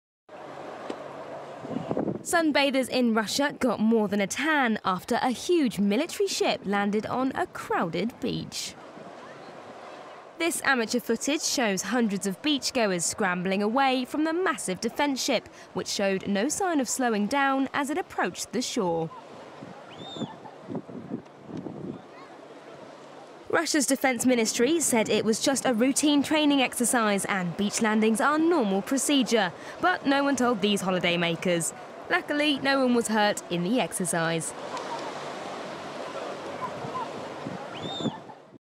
Russian military hovercraft ploughs into crowded beach - ITN Video
Video images belongs to ITN - EXCLUSEVELY.

Sunbathers in Kaliningrad are left bemused as a Russian military vessel makes an unannounced beach landing. Amateur footage shows holidaymakers scattering as the vessel approaches the shore, seemingly without slowing down. Russia's defence ministry says the crew were carrying out a routine training exercise.